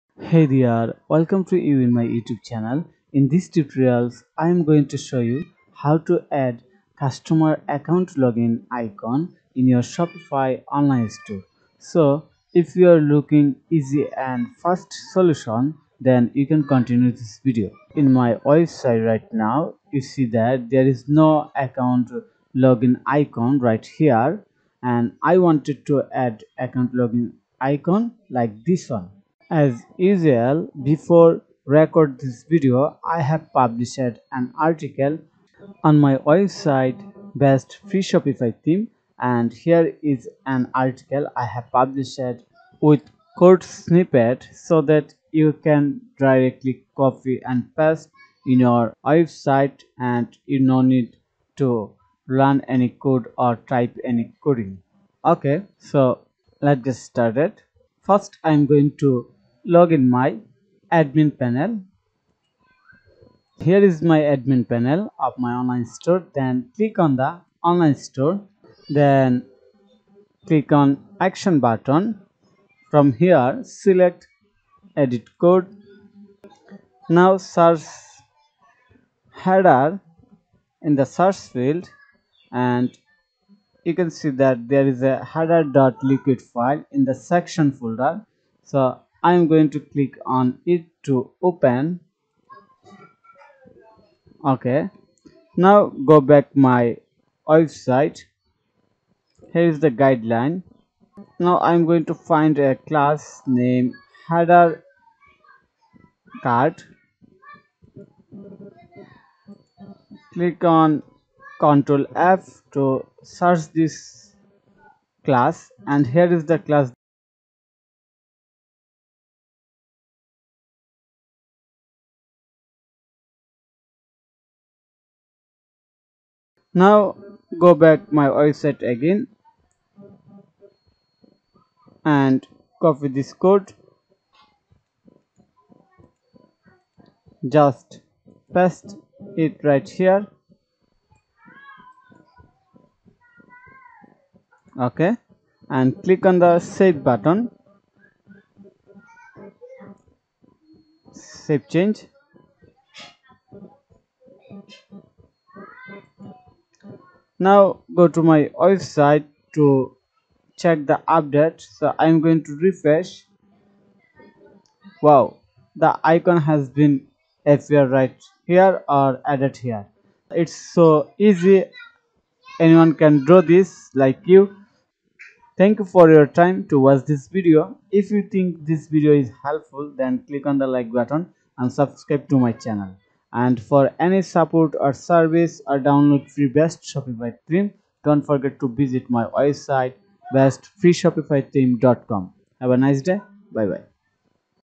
How to Add Customer Account Login Icon on Shopify in 2025 ✅ Easy & Fast
💻 Welcome to * How to Add Customer Account Login Icon in Shopify  ✅ Shopify Tutorial for Beginners  *